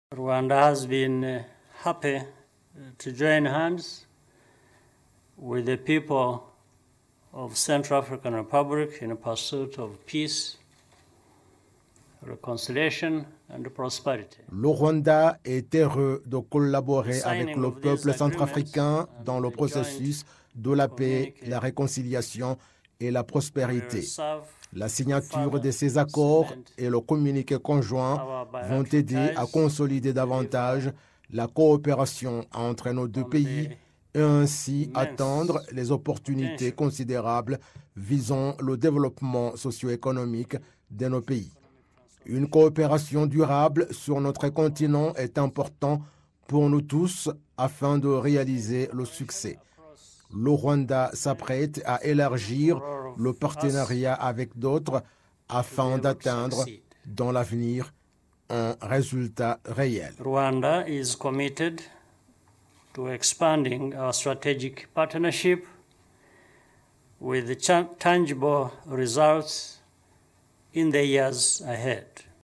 Abanyagihugu b'Urwanda na Centrafrika baramaze kubona ineza y'imigenderanire myiza
Urugendo rw’umukuru w’igihugu ca Centrafrika mu gihugu c’Urwanda ngo ni ikimenyetso gikomeye cerekana ko ivyo bihugu bibiri bimaze gushika kure mu bijanye n’imigenderanire. Paul Kagame, umukuru w’igihugu c’Urwanda, akaba yemeza ko ugufashanya arivyo bizotuma Afrika itera imbere.